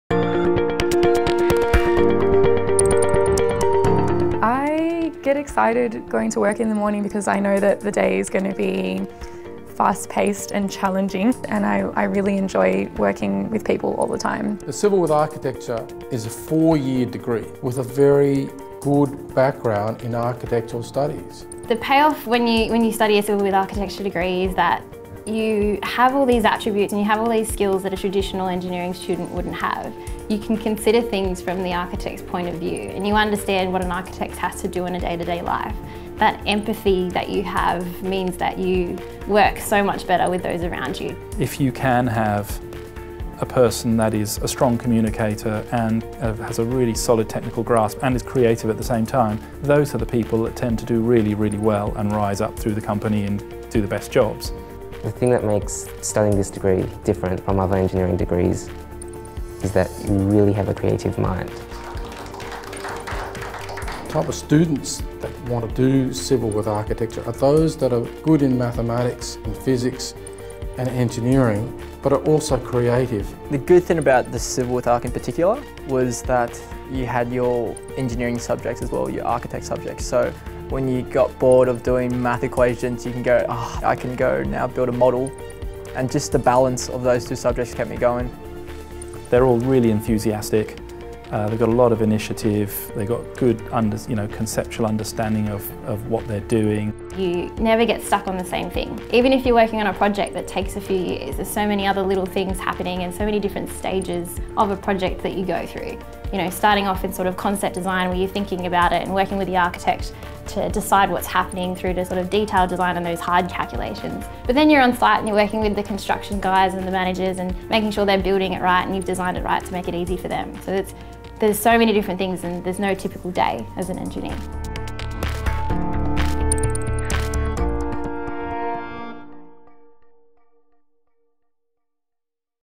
UNSW Civil with Architecture Degree
Promoting and informing prospective students about our undergraduate degree of civil engineering with architecture.
Showing its purpose and its career promise.